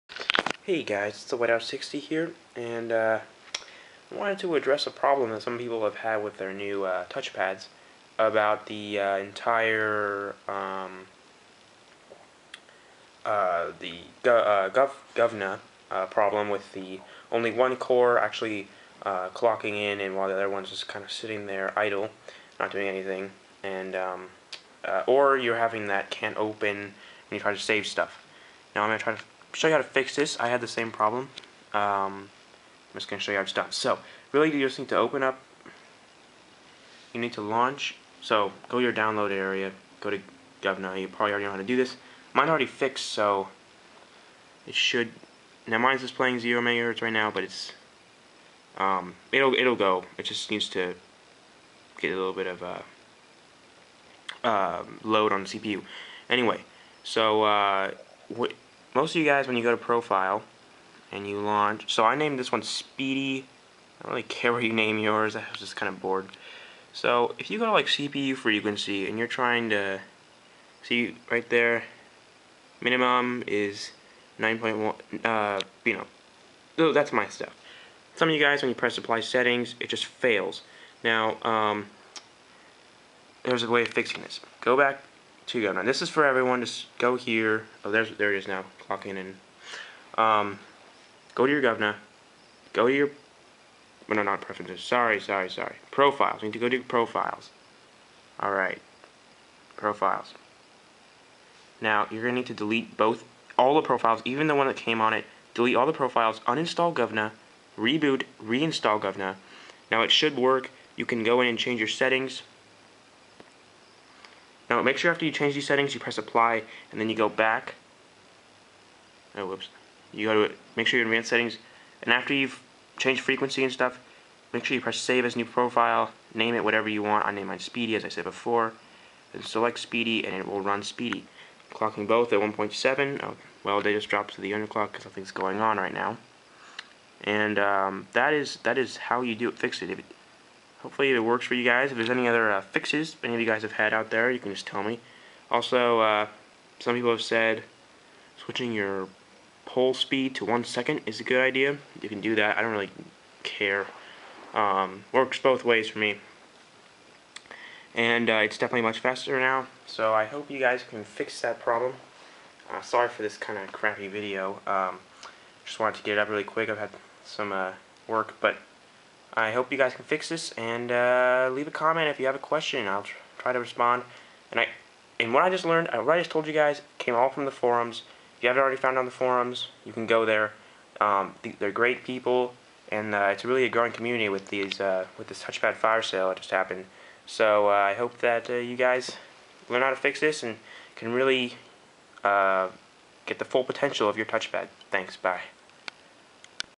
How to fix Govnah problems or get CPU2 to clock on Touchpad
This is a simple video on how to get the second cpu to clock in or to fix the unable to open error.

1) Delete ALL profiles.
2) uninstall govnah
3) reboot
4) reinstall
5) your done!